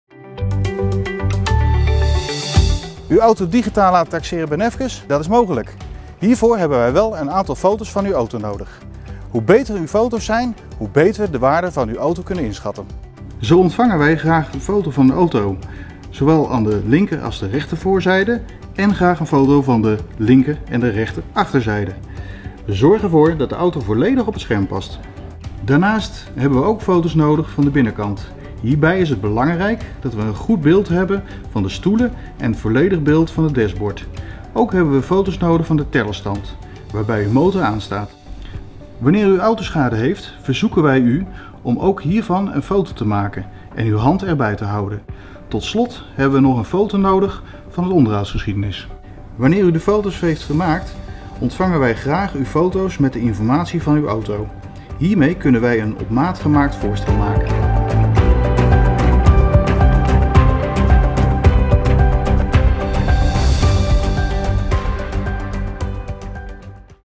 Zo laat u makkelijk en snel uw auto taxeren!
Wilt u uw huidige auto inruilen of aan ons verkopen? Ontvang snel een passend inruilvoorstel of bod. Het enige wat u hoeft te doen, is een aantal duidelijke foto's maken. In deze video leggen we uit hoe u die foto's kunt maken.